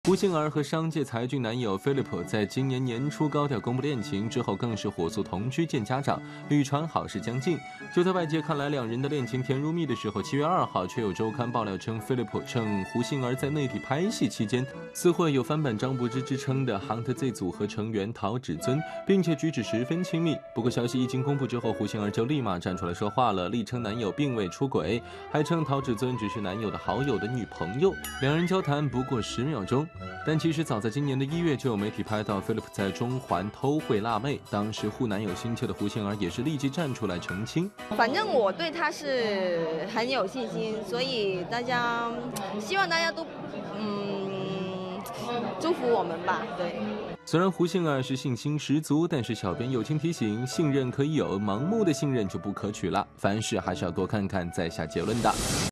《看看星闻》: 胡杏儿Myolie Wu否认男友夜店偷情Kankan News【SMG新闻超清版】
据香港“明报”报道，今年1月，胡杏儿于承认与Philip Lee恋爱，两人感情甜蜜，还被传年底就要结婚，但胡杏儿否认该消息，表示近半年要在内地拍戏。近日，有报道称，身为酒吧老板的Philip在夜店跟女模特咬耳朵，表现亲热。对男友的绯闻，胡杏儿表达了信任。